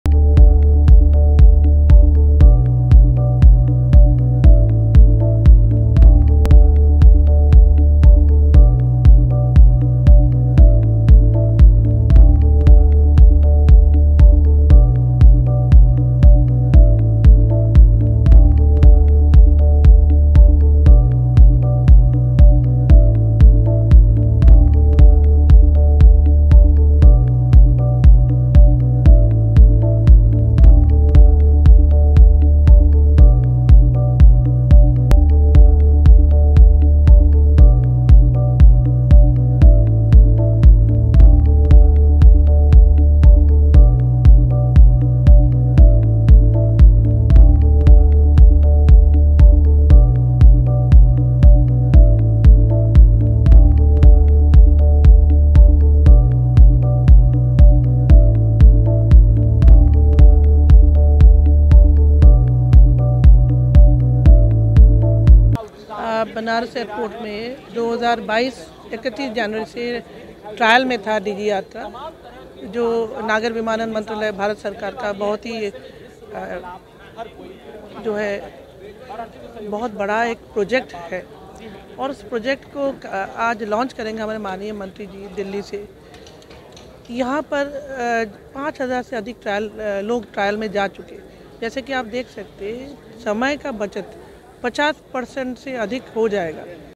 Varanasi: एयरपोर्ट पर बोर्डिंग पास के लिए नही करना होगा इंतजार,यात्री का चेहरा ही होगा बोर्डिंग पास
Varanasi: बोर्डिंग पास के लिए नही करना होगा वेट
यात्री का चेहरा ही होगा बोर्डिंग पास
चंद मिनटों में ही ऑनलाइन ऐप के
जरिए करा सकेंगे रजिस्ट्रेशन#up